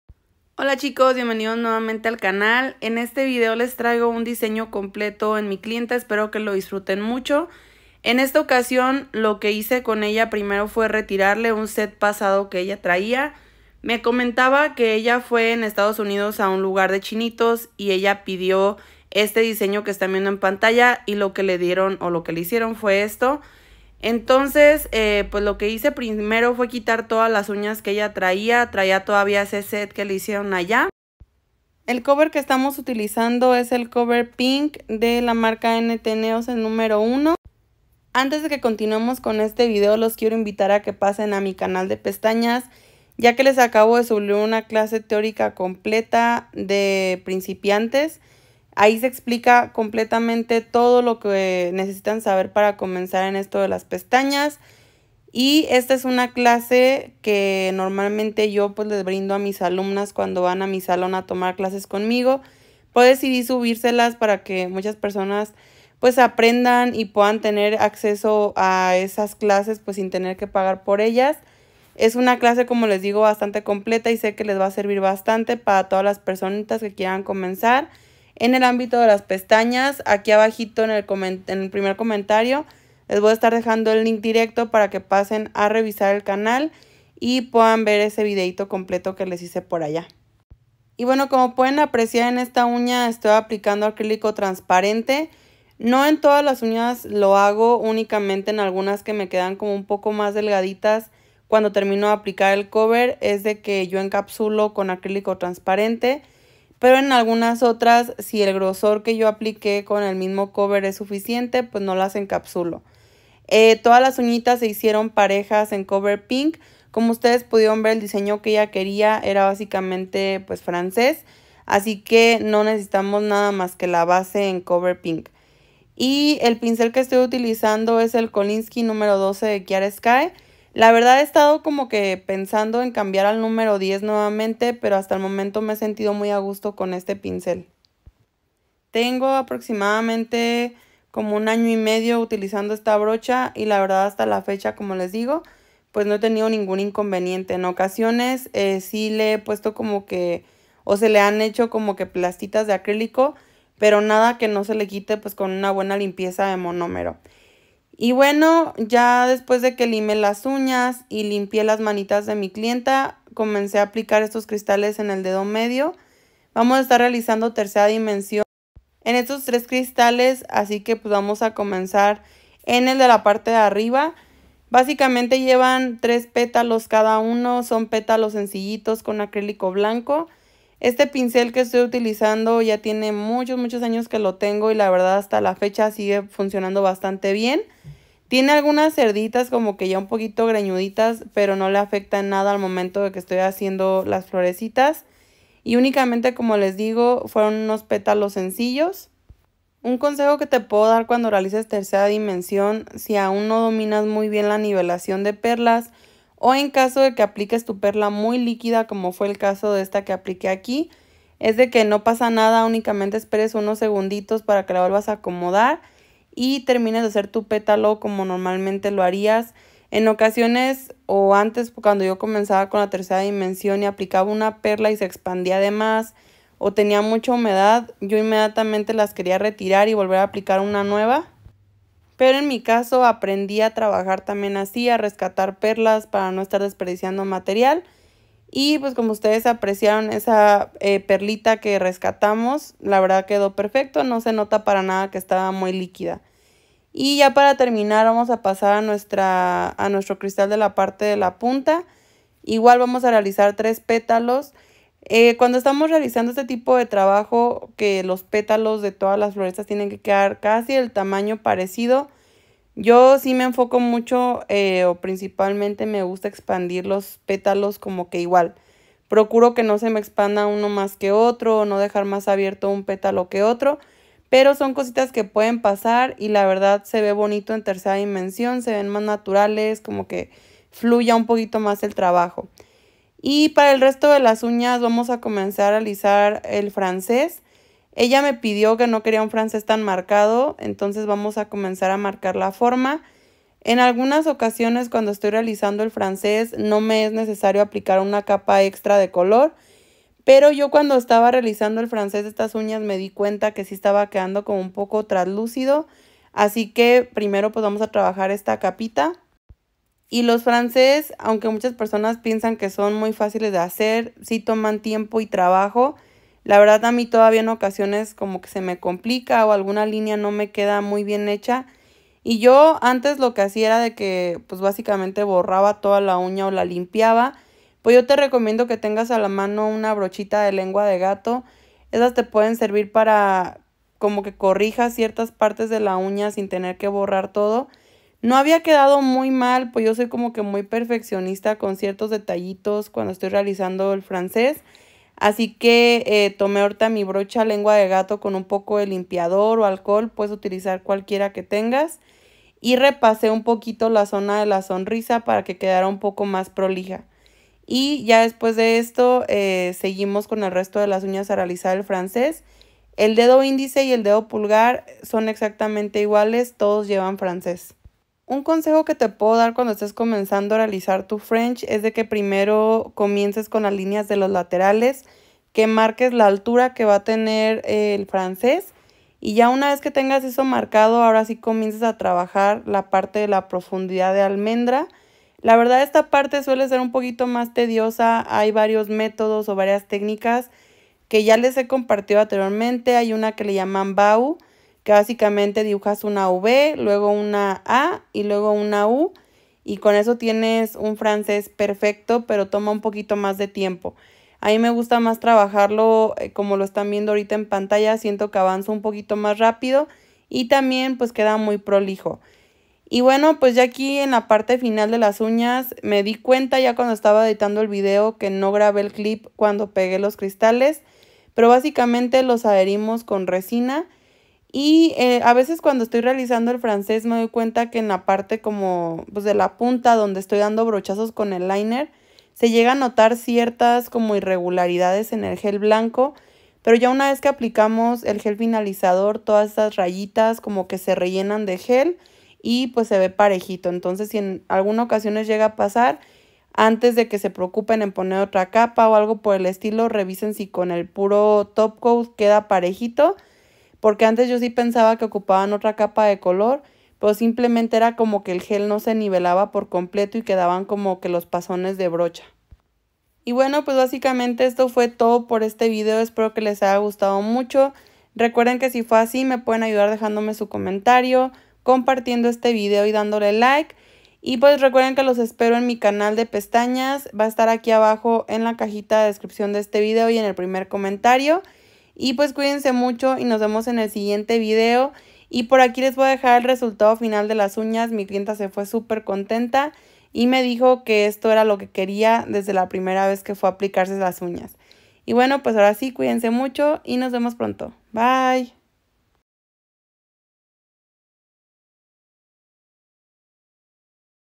MI CLIENTA PIDIO ESTÁS UÑAS Y QUEDARON MAL / Uñas acrílicas naturales / Jaayart
***Declaración de contenido**
Algunos productos mostrados en los vídeos pudieron recibidos como regalos, intercambios o partes de una colaboración, no estoy siendo pagada por ninguna compañía.